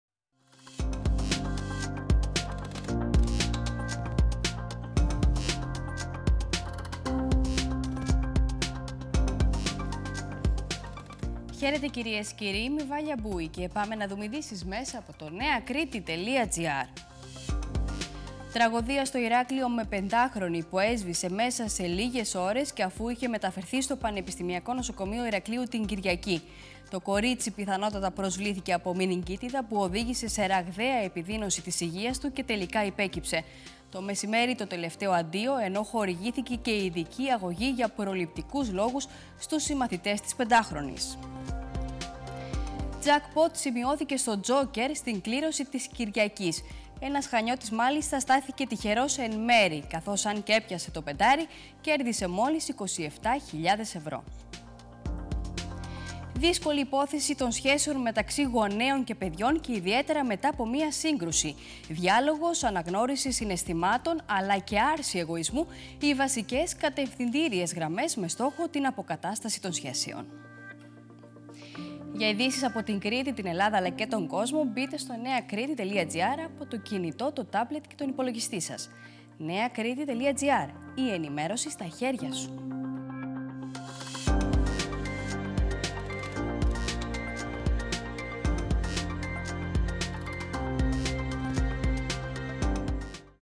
Σύντομη ενημέρωση από το neakriti.gr
Θάνατος 5χρονης: Τα πρώτα στοιχεία για τα αίτια θανάτου

Στην Κρήτη παίχτηκε το τυχερό 5άρι του Τζόκερ

Όταν οι σχέσεις σας με τα παιδιά σας ταλανίζονται...

Με τη Βάλια Μπούη